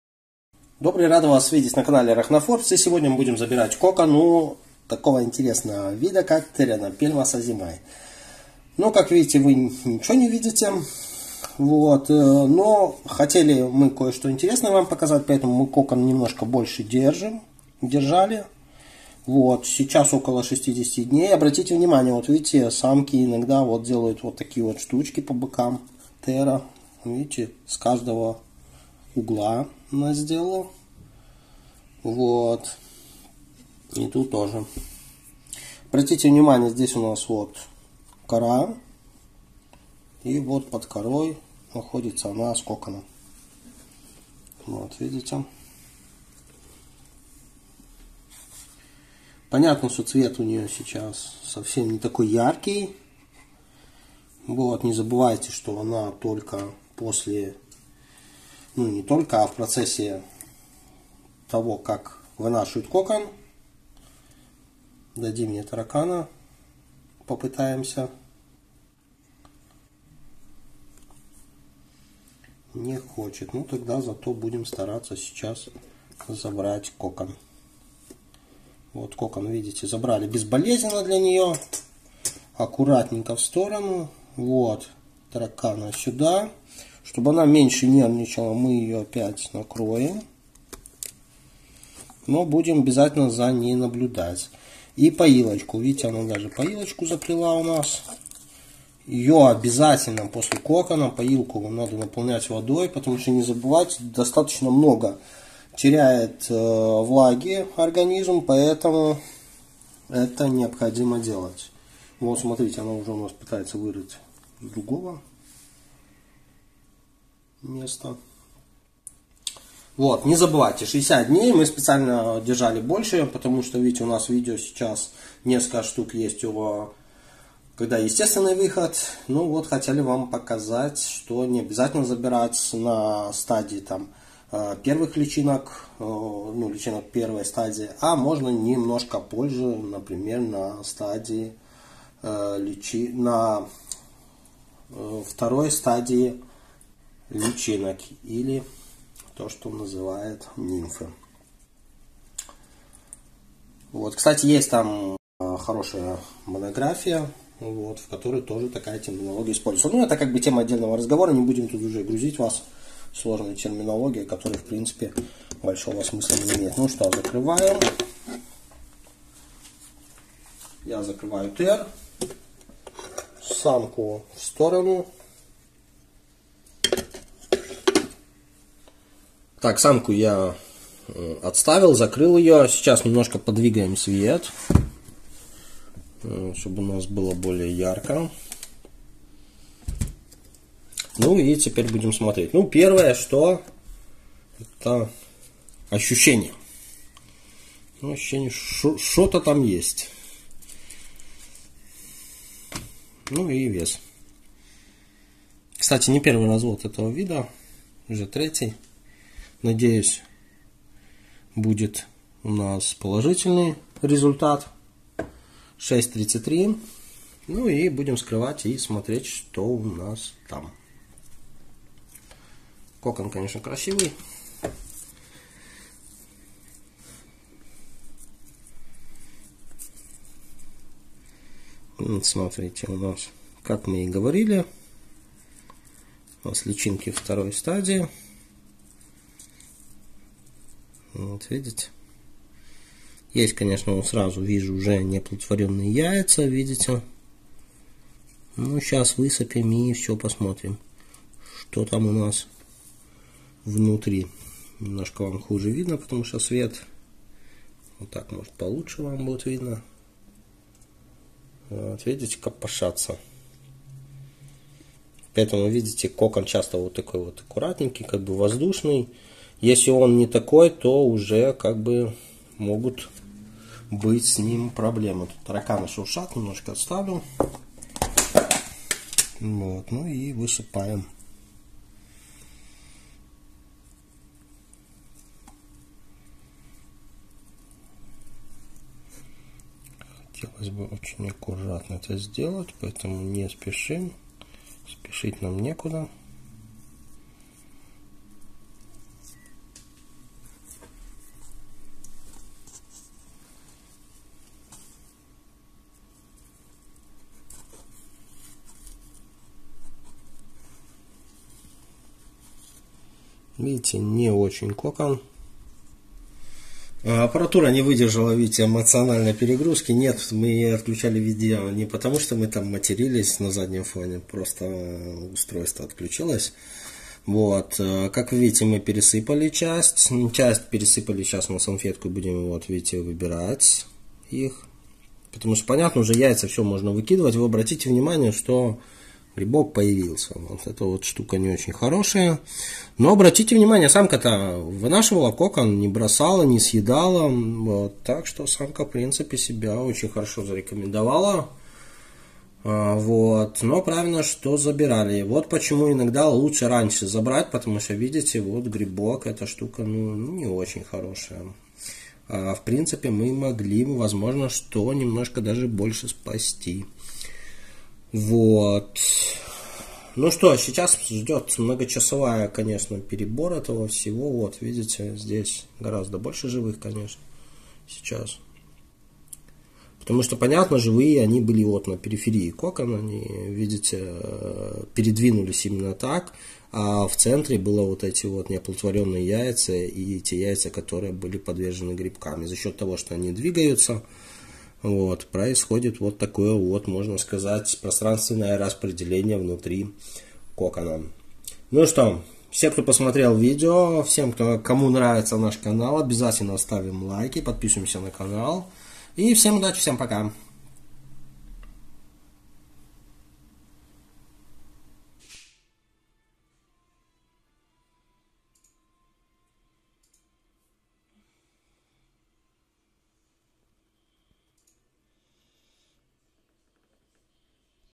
Красивый паук птицеед сделал кокон Lasiocyano sazimai (ex. Pterinopelma sazimai)
Красивый паук птицеед сделал кокон Pterinopelma sazimai
Pterinopelma sazimai  назван в честь доктора Иван Сазима, бразильский зоолог, который первым открыл этот вид в 1971 году и исследовал его в течение 10 лет вместе с Сильвией Лукас. Вид были обнаружены на большой высоте с плохой растительностью и экстремальными погодными условиями (15-35 ° C), переживая периоды сильных осадков и засухи. Обратите внимание, что паук защищает себя от экстремальных погодных условий, создавая толстую паутину под и над стволами деревьев, камнями, ветвями и листьями.

Помочь проекту Тинькофф.
Номер карты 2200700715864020 до 12/30
Получатель: Д. Виктор Викторович